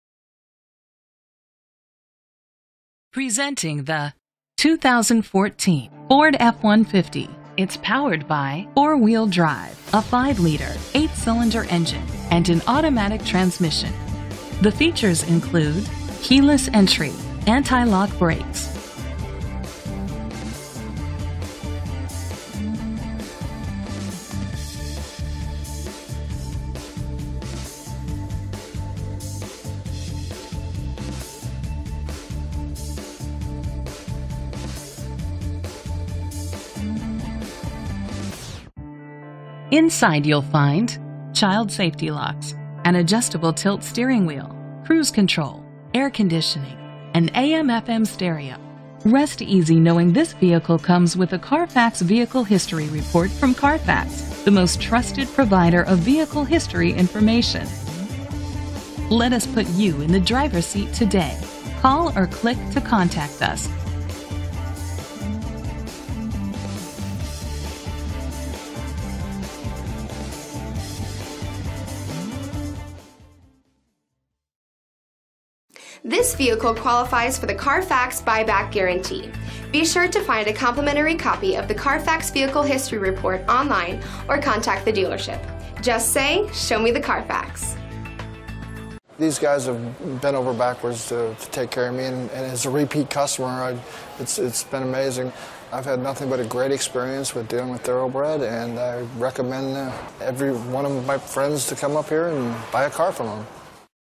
2014 Ford F-150 61503A - Kansas City MO
Year: 2014
Make: Ford
Model: F-150
Trim:
Engine: 5.0 liter 8 Cylindercylinder 4WD
Transmission: Automatic
Color: White
Mileage: 36904
Address:
8501 North Boardwalk Ave
Kansas City, MO 64154
VIN:1FTFW1EF1EFC35488